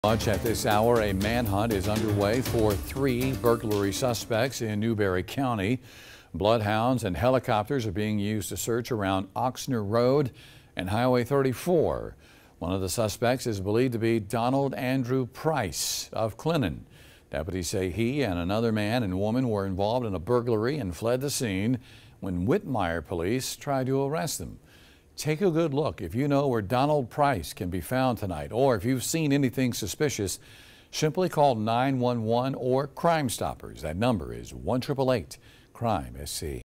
Manhunt underway in Newberry county
A man hunt is underway for three burglary suspects in Newberry County.  Bloodhounds and helicopters are being used to search around Oxner Road and Highway 34.